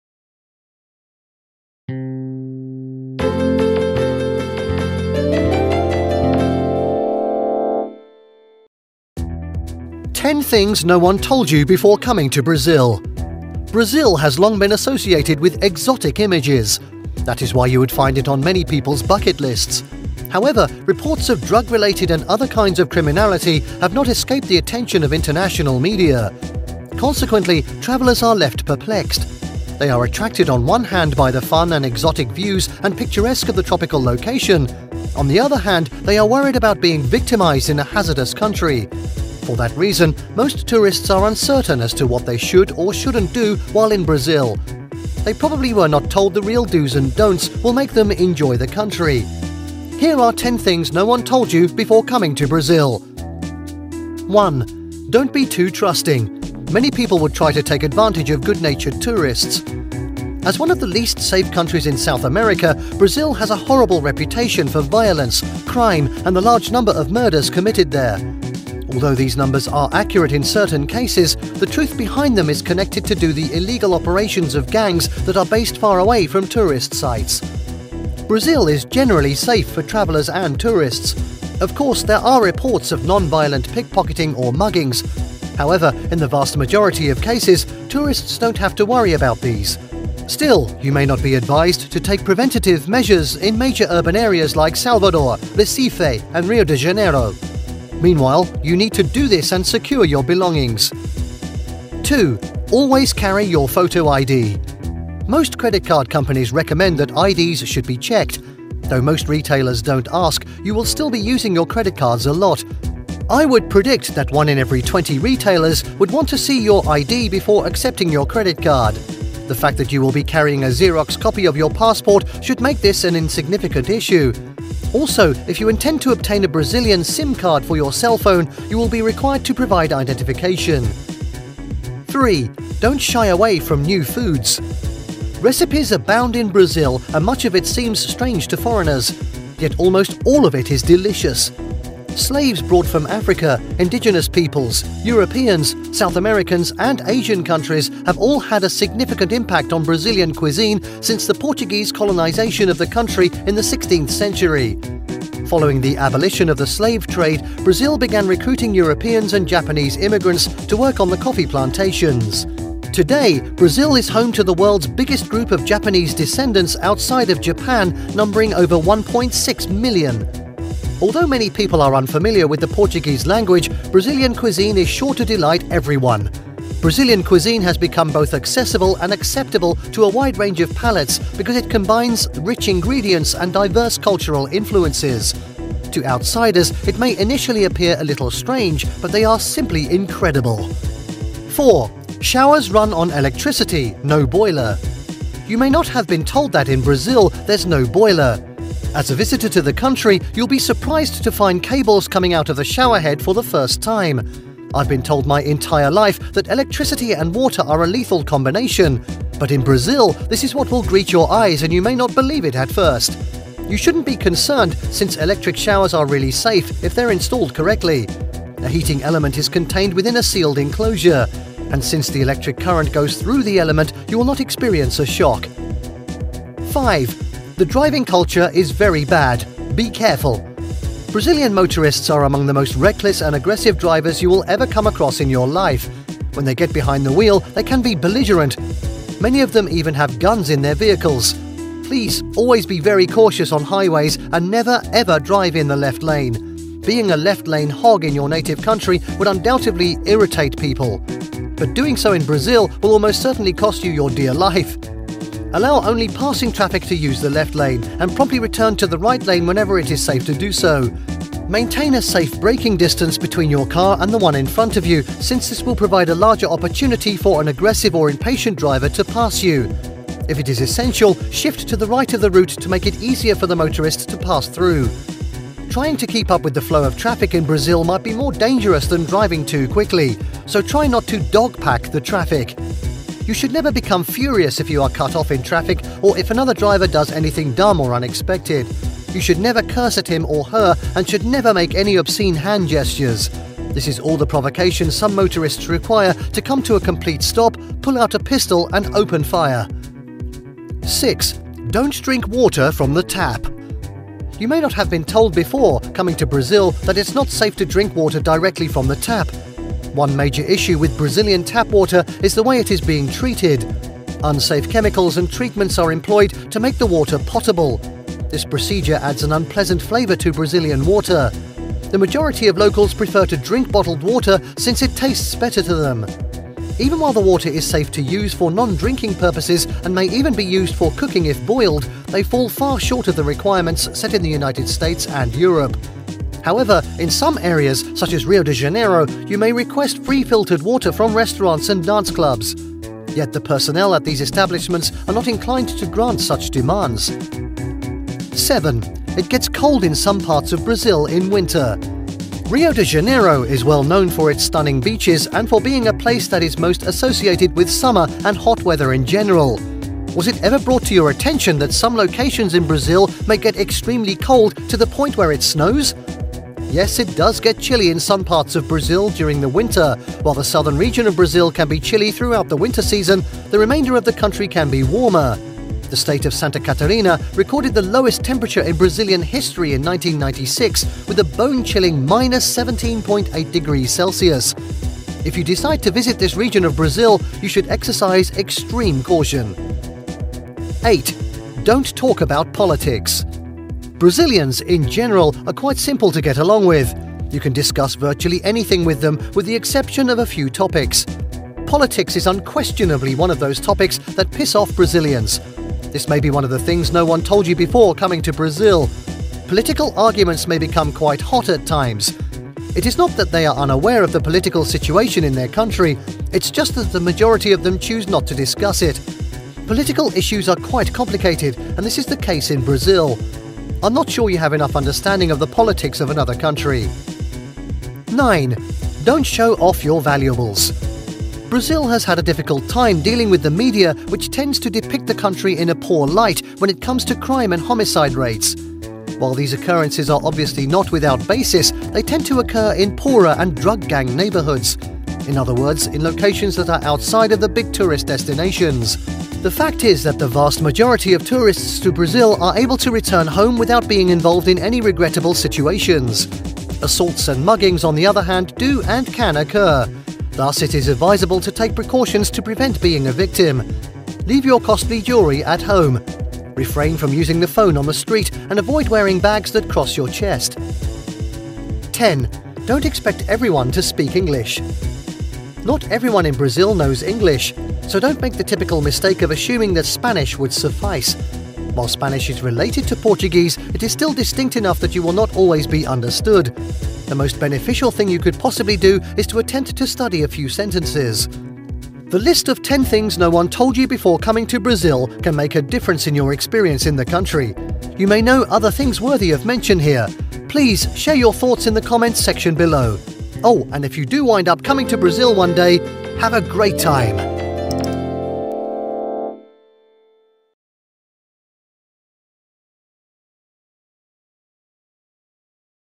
10 Things No One Told You Before Coming to Brazil
Brazil has long been associated with exotic images. That is why you would find it on many people’s bucket lists. However, reports of drug-related and other kinds of criminality have not escaped the attention of international media. Consequently, travelers are left perplexed. They are attracted on one hand by the fun and exotic views and picturesque of the tropical location. On the other hand, they are worried about being victimized in a hazardous country. For that reason, most tourists are uncertain as to what they should or shouldn't do while in Brazil. They probably were not told the real dos and don’ts will make them enjoy the country. Here are 10 things no one told you before coming to Brazil.
1. Don’t be too trusting. Many people would try to take advantage of good-natured tourists
As one of the least safe countries in South America, Brazil has a horrible reputation for violence, crime, and the large number of murders committed there. Although these numbers are accurate in certain cases, the truth behind them is connected to do the illegal operations of gangs that are based far away from tourist sites.
Brazil is generally safe for travelers and tourists. Of course, there are reports of non-violent pickpocketing or muggings. However, in the vast majority of cases, tourists don’t have to worry about these. Still, you may not be advised to take preventive measures in major urban areas like Salvador, Recife, and Rio de Janeiro. Meanwhile, you need to do this and secure your belongings.
2. Always carry your photo ID
Most credit card companies recommend that IDs should be checked. Though most retailers don’t ask, you will still be using your credit cards a lot. I would predict that one in every twenty retailers will want to see your ID before accepting your credit card. The fact that you will be carrying a xerox copy of your passport should make this an insignificant issue. Also, if you intend to obtain a Brazilian SIM card for your cell phone, you will be required to provide identification.
3. Don’t shy away from new foods.
Recipes abound in Brazil and much of it seems strange to foreigners. Yet, almost all of it is delicious. Slaves brought from Africa, indigenous peoples, Europeans, South Americans, and Asian countries have all had a significant impact on Brazilian cuisine since the Portuguese colonization of the country in the 16th century. Following the abolition of the slave trade, Brazil began recruiting Europeans and Japanese immigrants to work on the coffee plantations. Today, Brazil is home to the world's biggest group of Japanese descendants outside of Japan numbering over 1.6 million.
Although many people are unfamiliar with the Portuguese language, Brazilian cuisine is sure to delight everyone. Brazilian cuisine has become both accessible and acceptable to a wide range of palates because it combines rich ingredients and diverse cultural influences. To outsiders, it may initially appear a little strange, but they are simply incredible.

Bitcoin:  1JvUtAS2MKJ3y9WghQmp9a9dKMxTfQUHhz
Doge Coin:  DLAiLVRAg3JrPiLxSwgcoBxgHzTg4Fq3W1
Ethereum:  0xdd897e1c8e76D90b343Aef1491fad93780890F82

Get stock footage for your videos: shorturl.at/gsEI6